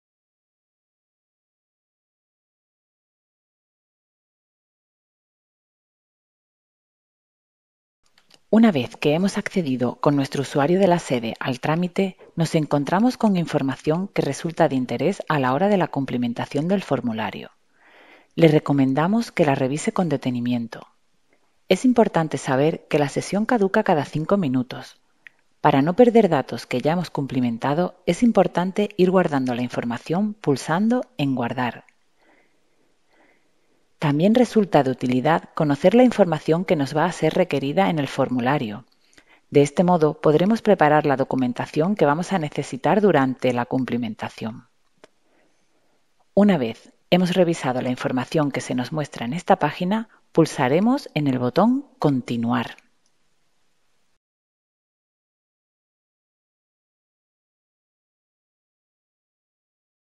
Información general (universidad)
Información de interés antes de cumplimentar el impreso en becas universitarias.